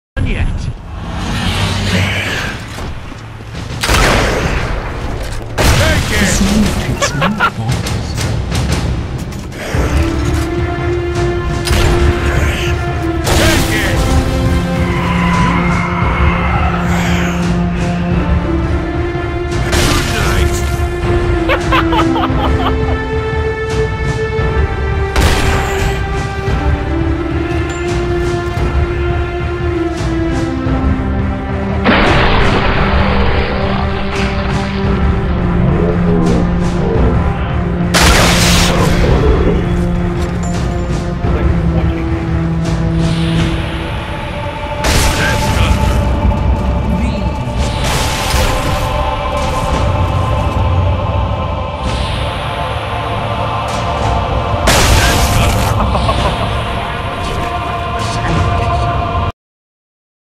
Terminhator returns
Three billion locust lives ended on November 20, 2015. The survivors of the nuclear fire called the war Judgminht Day. They lived only to face a new nightmare: the war against the minhchines. The computer which controlled the minhchines, Botnet, sent two Terminhators back through time. Their mission: to destroy the leader of the locust resistance, RAAM, my son. The first Terminhator was programmed to strike at me in the year 2008, before RAAM was born. It failed. The second was set to strike at RAAM himself when he was still a grublet. As before, the resistance was able to send a lone warrior, a protector for RAAM. It was just a question of which one of them would reach him first.